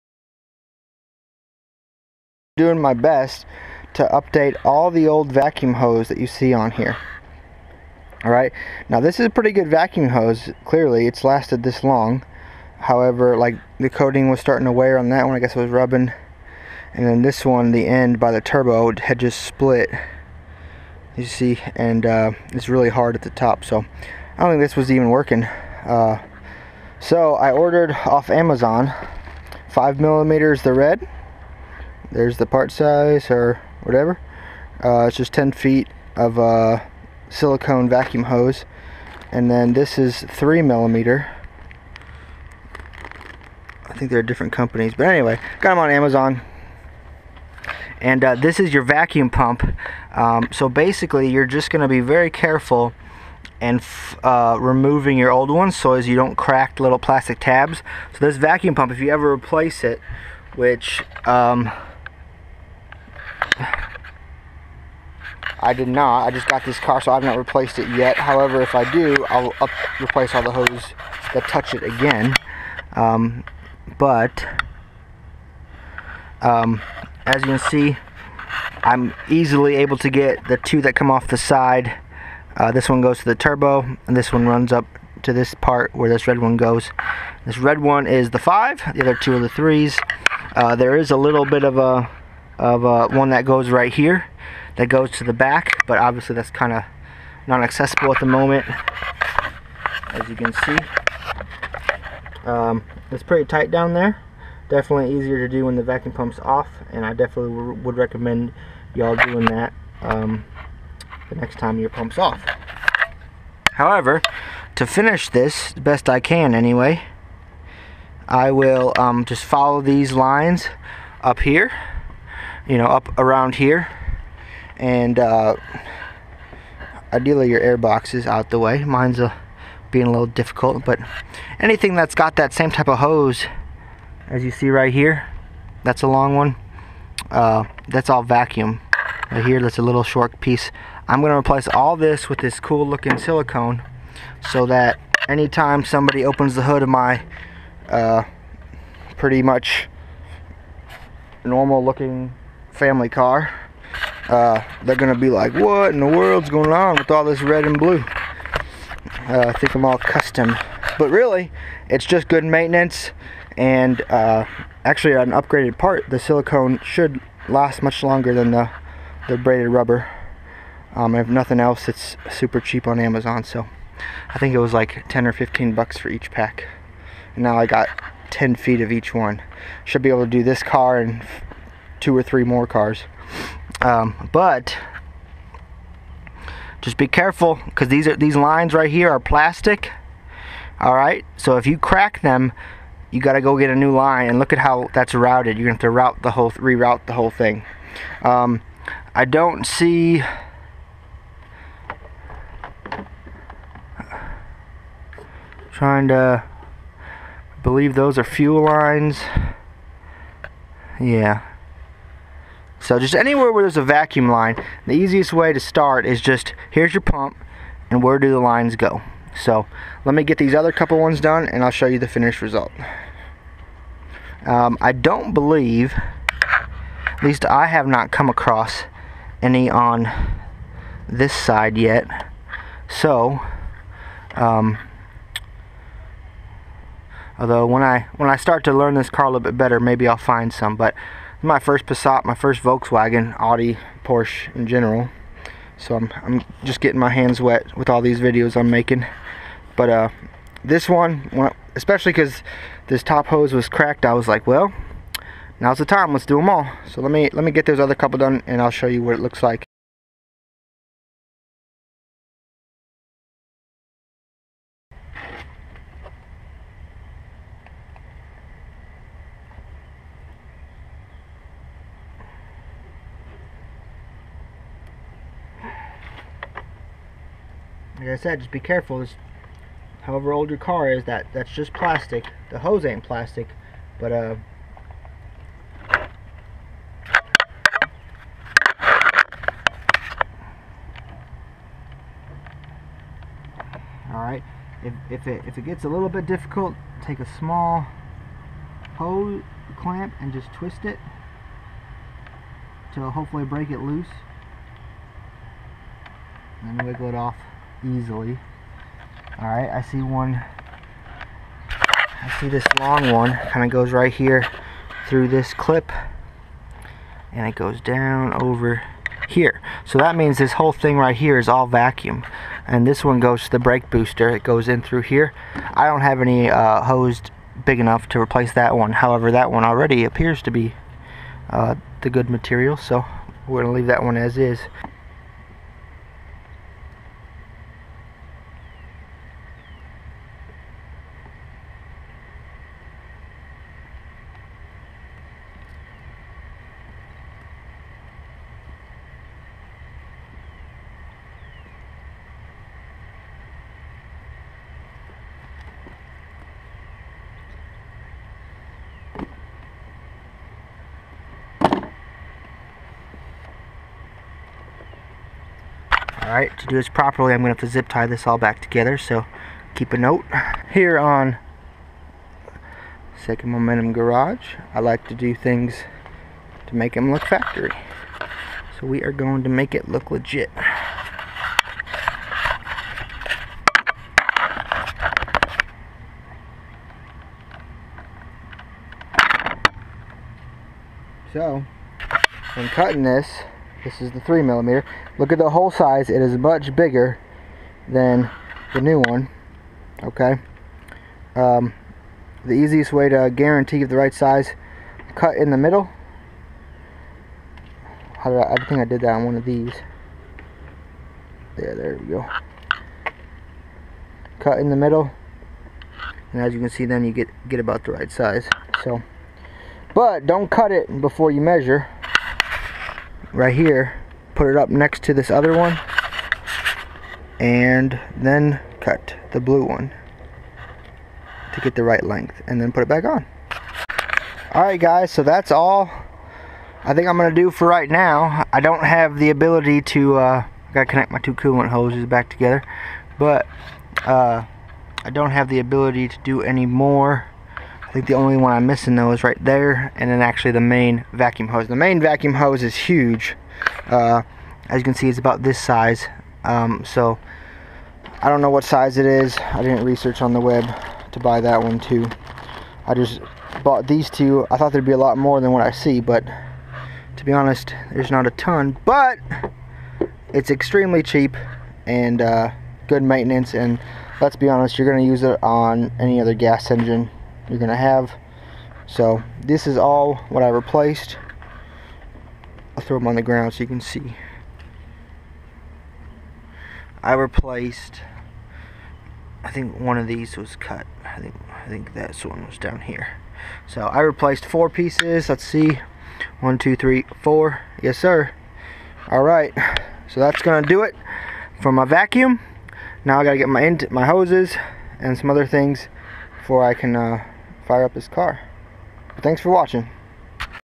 2005 Passat Vacuum Hose replacement
My 2005 passat is 16+ years old and had 155k miles. Some of the vacuum hoses were showing their age and I decided to grab some 3mm and 5mm vacuum hose off amazon to replace when I could get to.

See more on instagram @2ndMomentumGarage